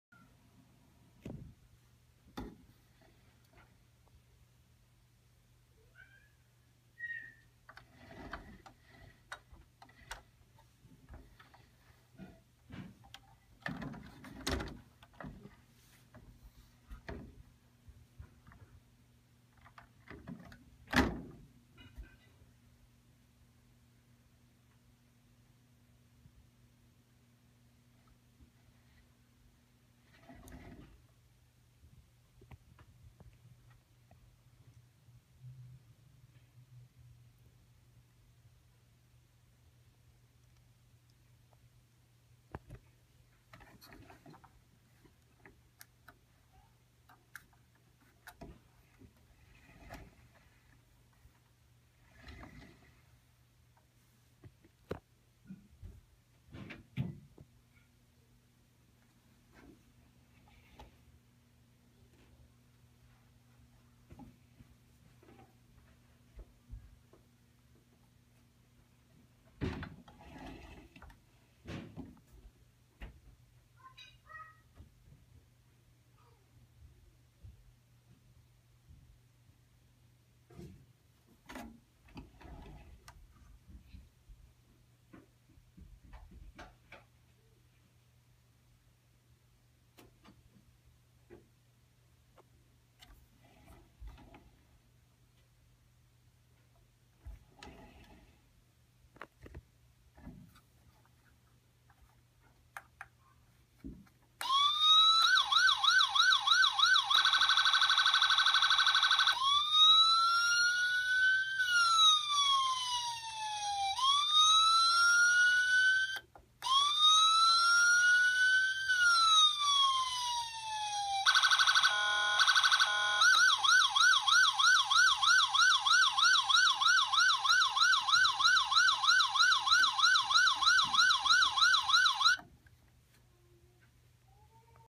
Kentucky State Police w/working lights
Lit dash, lit top, lit radar and siren